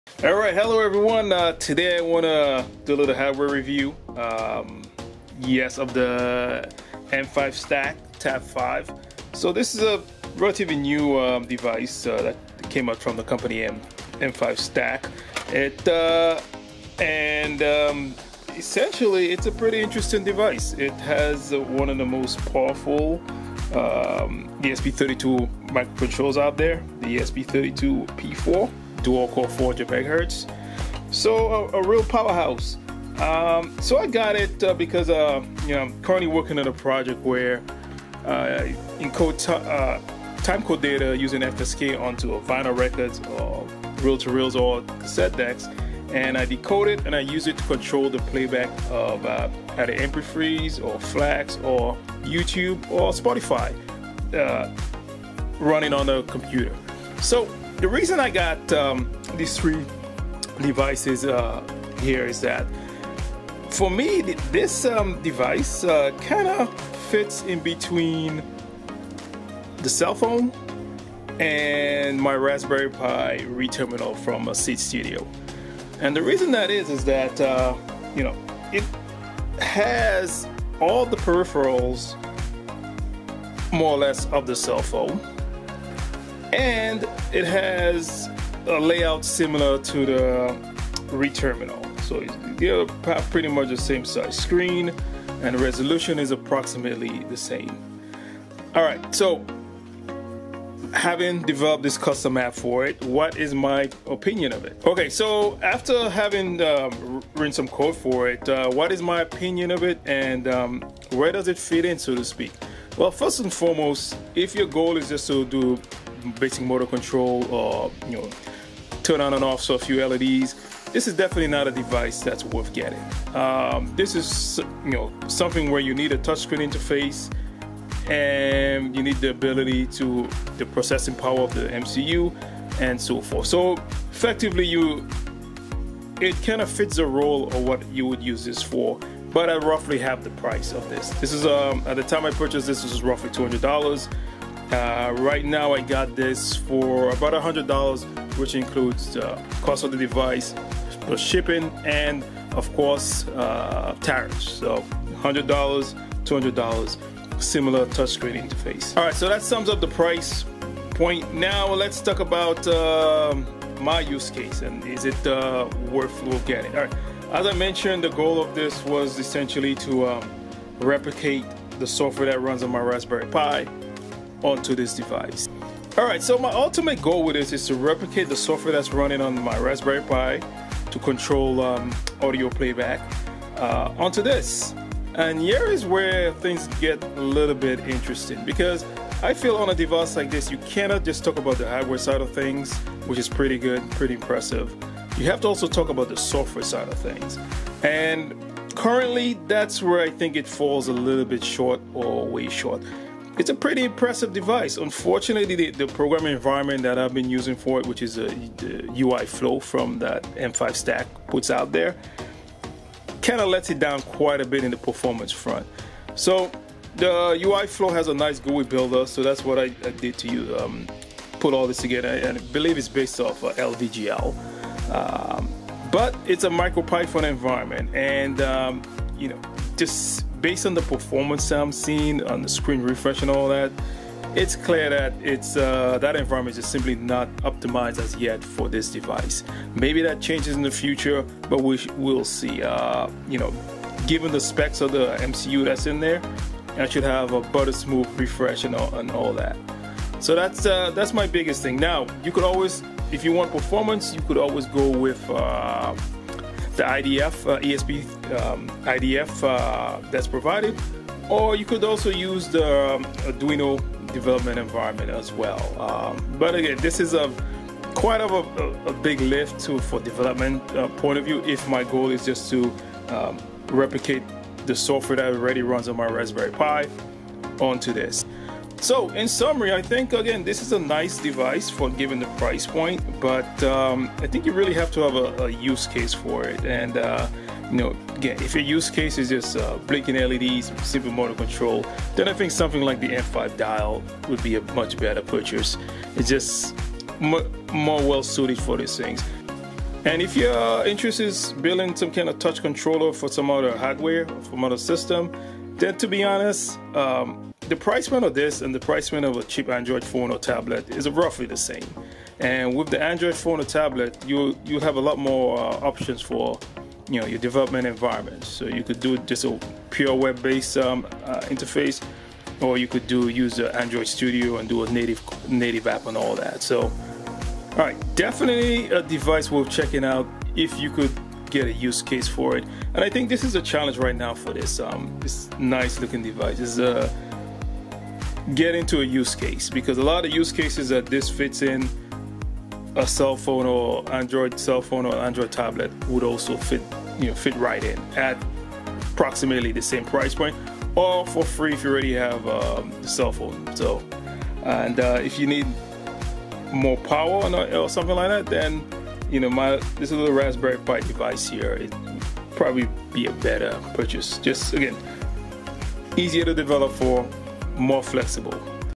M5Stack Tab5 Review
First look at the M5Stack Tab5 for use as an FSK Encoder/Decoder music playback application. It's compared to Seeed Studio's ReTerminal and Android phone for the same application.